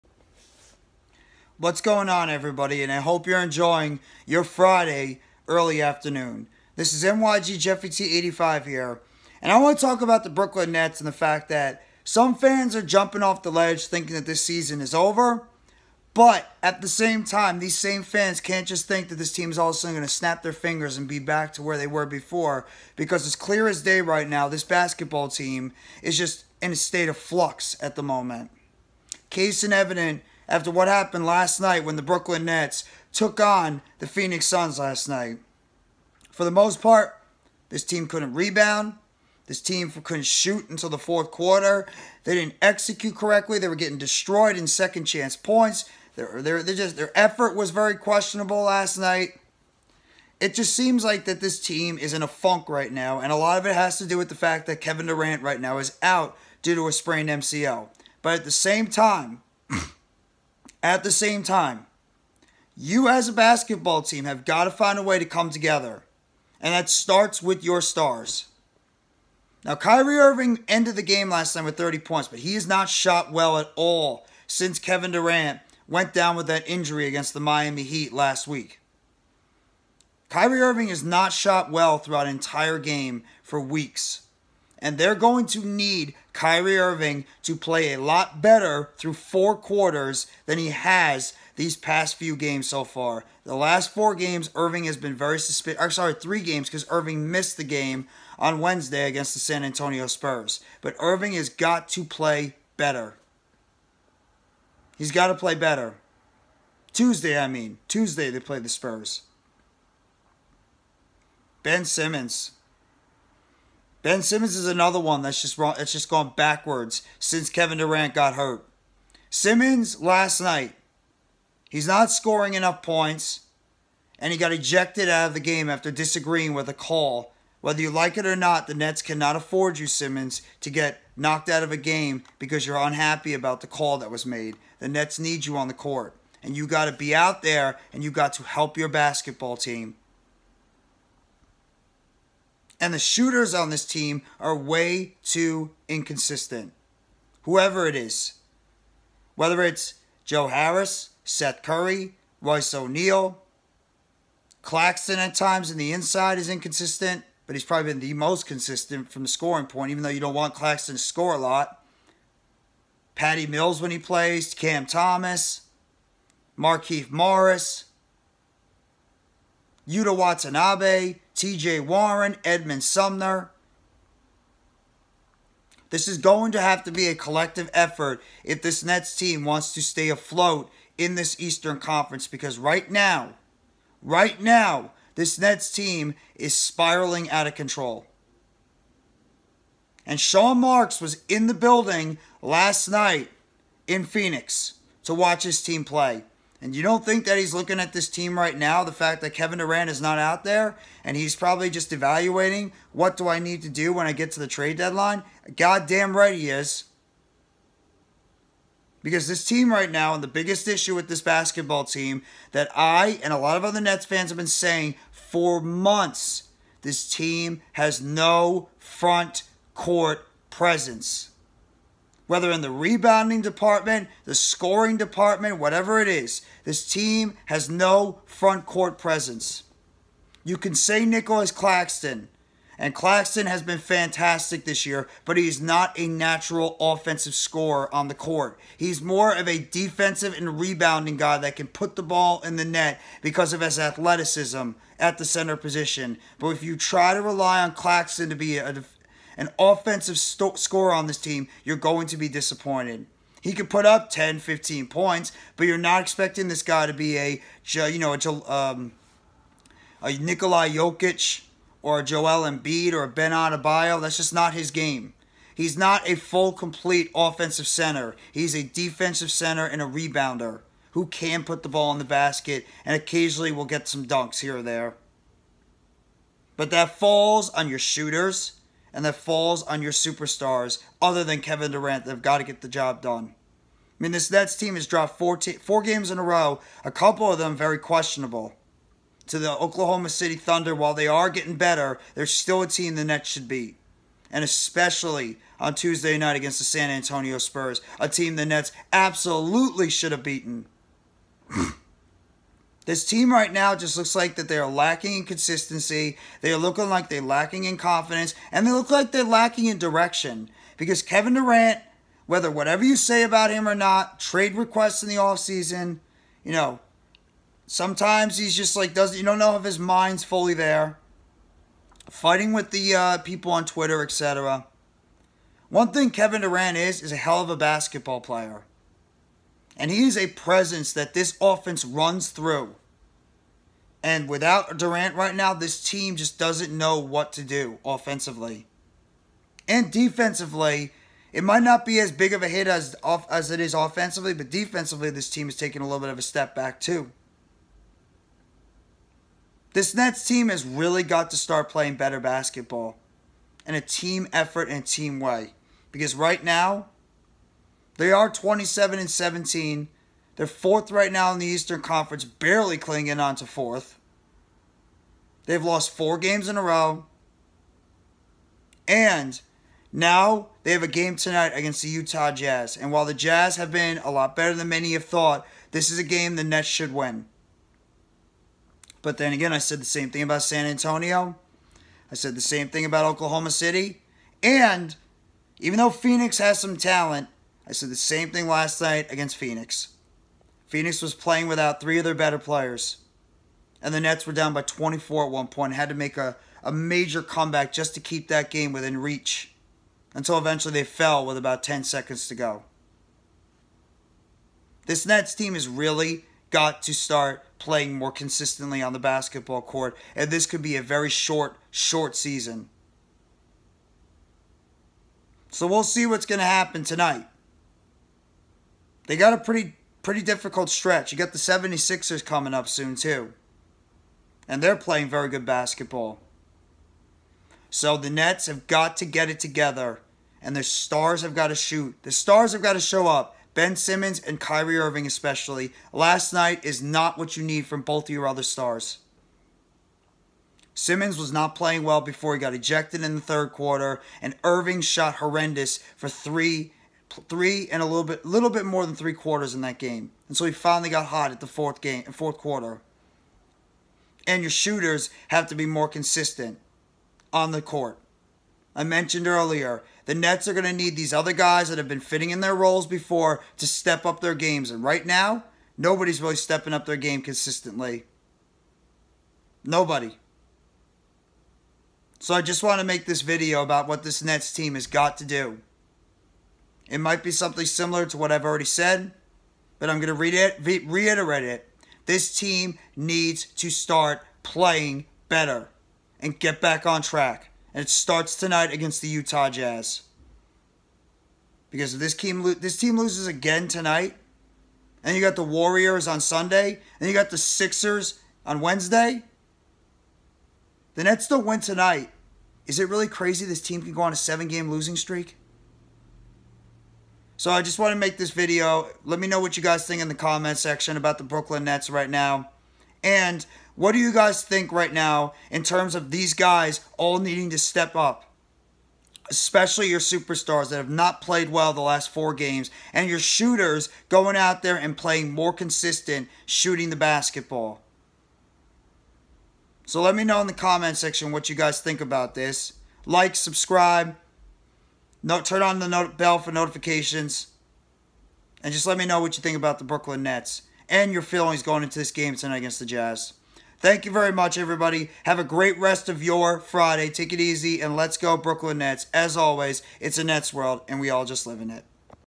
Nets Undercover: Irving/Simmons need to raise their games in Durant’s absence
The Brooklyn Nets are in need of their two other star players and more consistent shooting from their role players while they are trying to weather the storm without their superstar small forward Kevin Durant and that will mostly fall on the play of Kyrie Irving and Ben Simmons to help lead the way for the Nets in Durant’s absence and unfortunately so far the Nets have gone 0-4 since Durant sprained his MCL last Sunday against the Miami Heat but the rest of their team namely their two other stars need to raise their game to help keep this team near the top of the Eastern Conference standings until Durant returns from injury sometime in February.